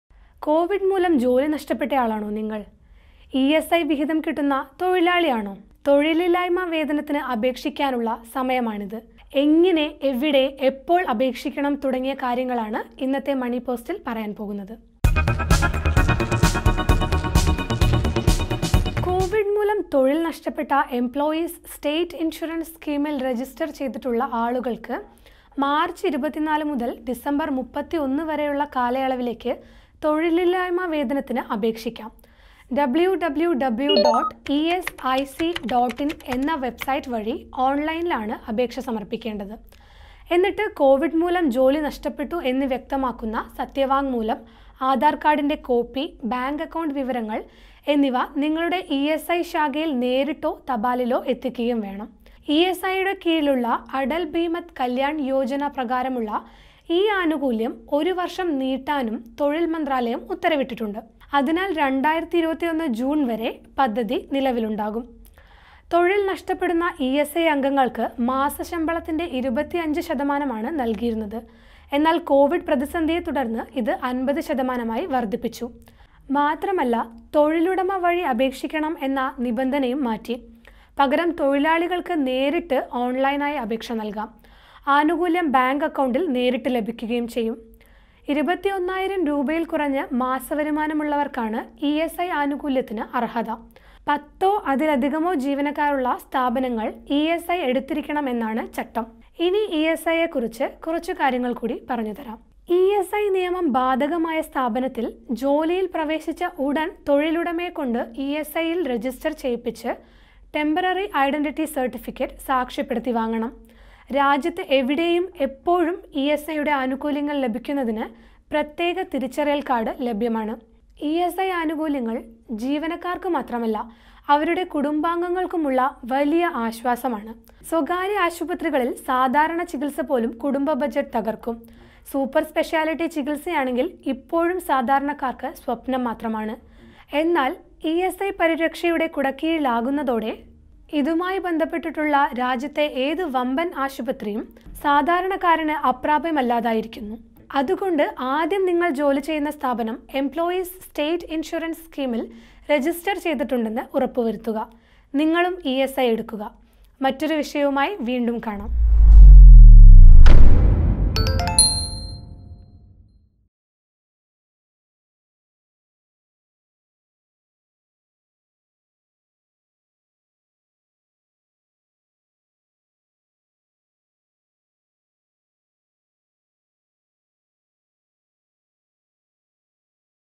കൊവിഡ് തൊഴില്‍ ഇല്ലാതാക്കിയോ? സഹായത്തിന് ഇപ്പോള്‍ അപേക്ഷിക്കാം | Muziriz Post
കോവിഡ് മൂലം ജോലി നഷ്ടപ്പെട്ട ആളാണോ നിങ്ങൾ? ഇഎസ്‌ഐ വിഹിതം കിട്ടുന്ന തൊഴിലാളിയാണോ? എങ്കിൽ, തൊഴിലില്ലായ്മ വേതനത്തിനു അപേക്ഷിക്കാനുള്ള സമയമാണിത്. എങ്ങിനെ? എവിടെ? എപ്പോൾ അപേക്ഷിക്കണം തുടങ്ങിയ കാര്യങ്ങളാണ് ഇന്നത്തെ മണി പോസ്റ്റിൽ പറയുന്നത്...

You can also follow MUZIRIZ POST's social media platforms and engage with us.